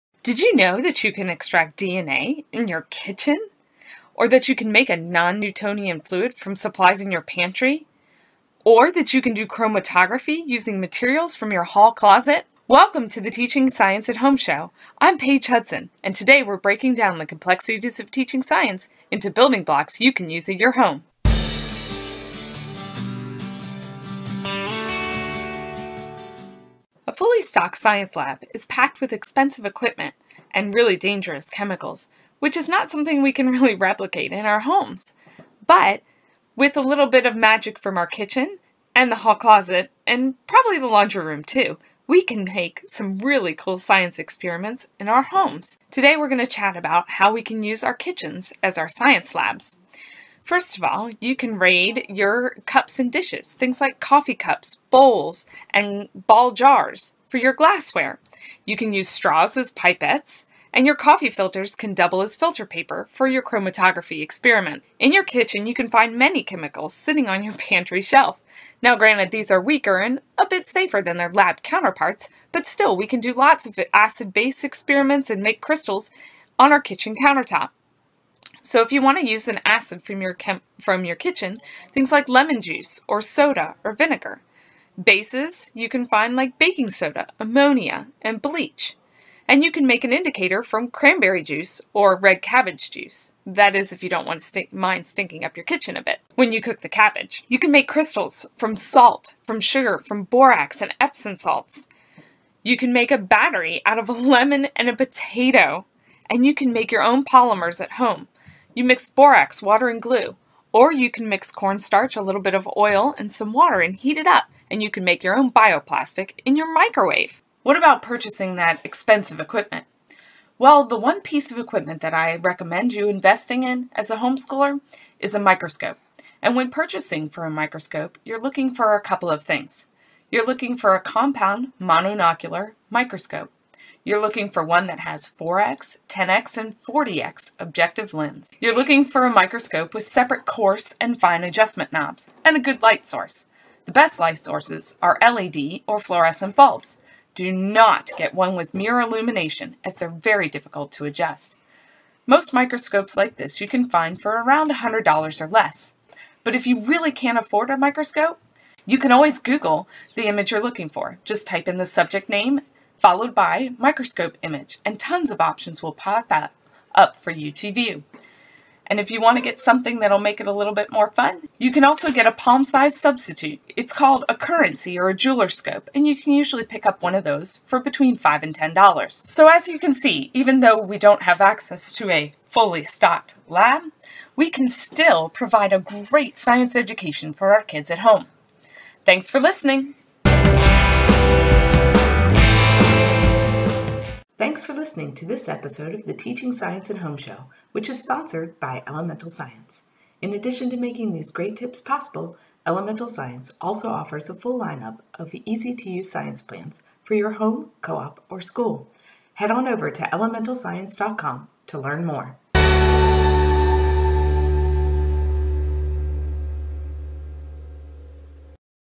The fully-stocked home science laboratory
Before you start teaching science at home, do you really have to buy a full set of glassware, chemicals, and equipment? In this episode, Paige is going to show you how you can have a fully-stocked home science laboratory without breaking the bank!

Don't miss a tip!